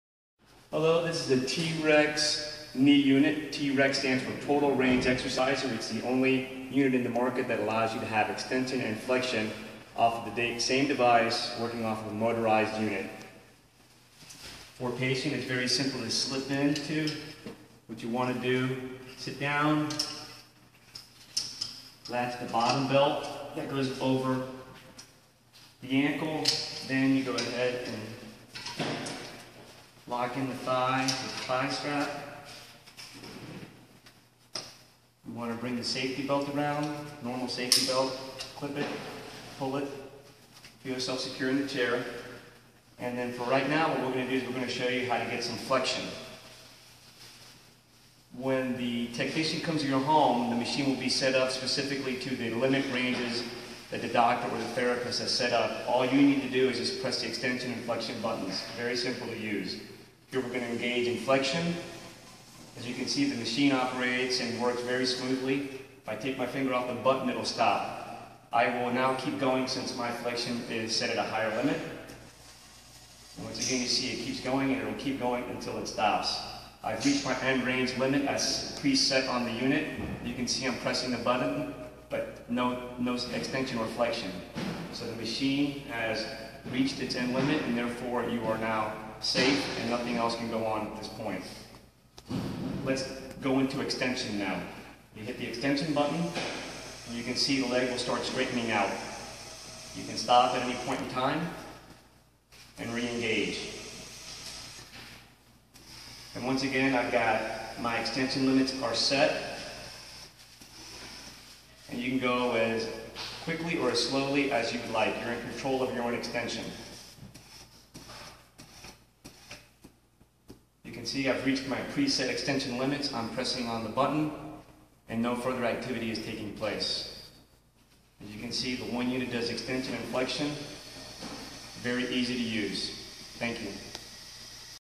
T-Rex Knee Unit - Total Range Exerciser
T-Rex Knee Unit - Total Range Exerciser, the only machine that allows you to extension and flexion with the same machine.

Rehabilitaiton Equipment for knee injuries.